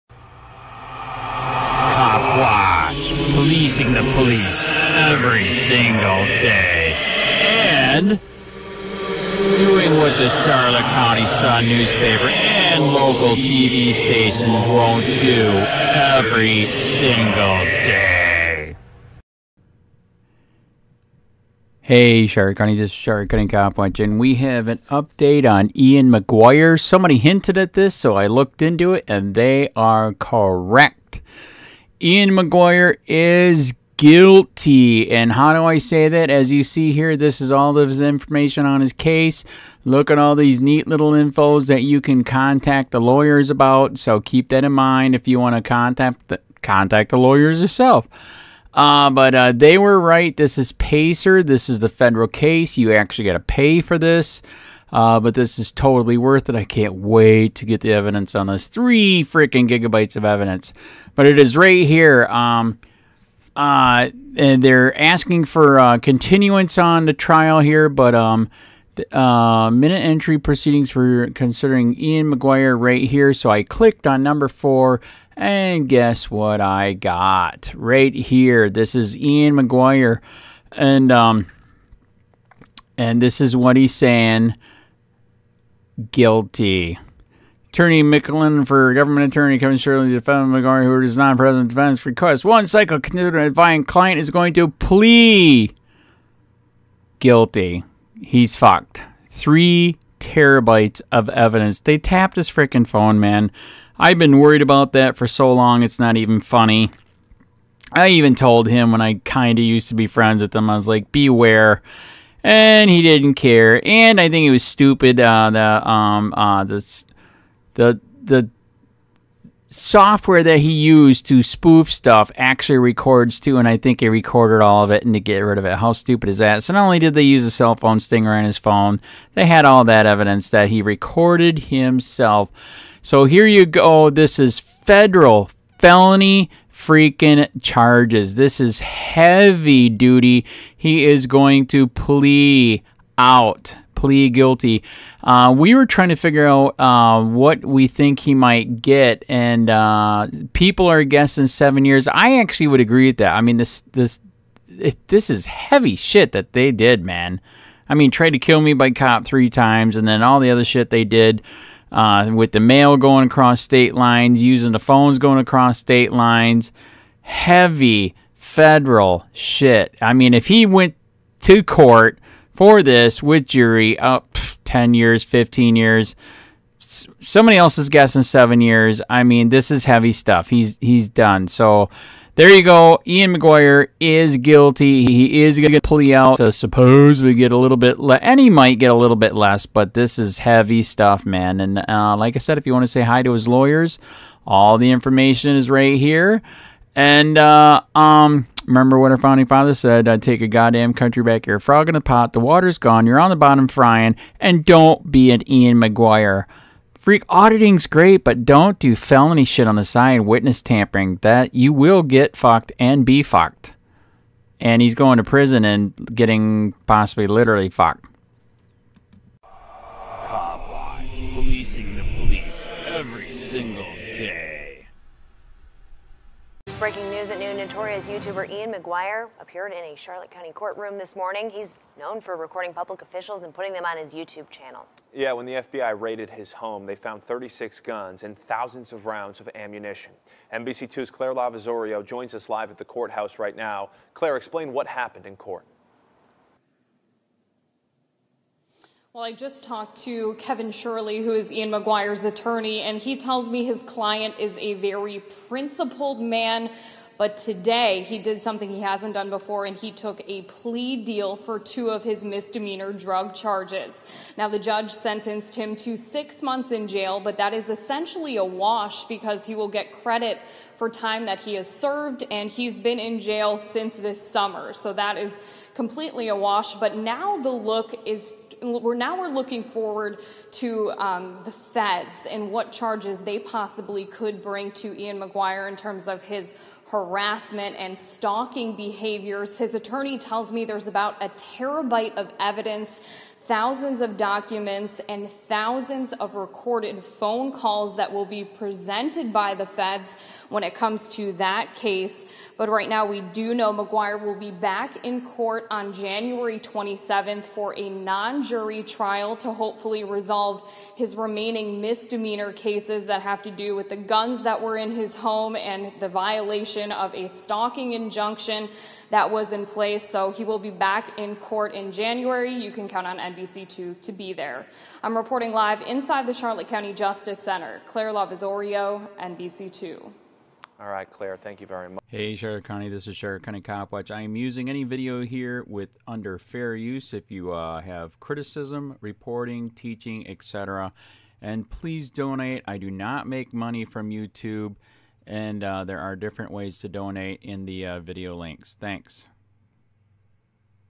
IAN MCGUIRE TAKING PLEA DEAL ON FEDERAL FELONY CHARGES MESSING WITH MAIL PHONE ACROSS STATE LINES'
AUDIT,COPWATCH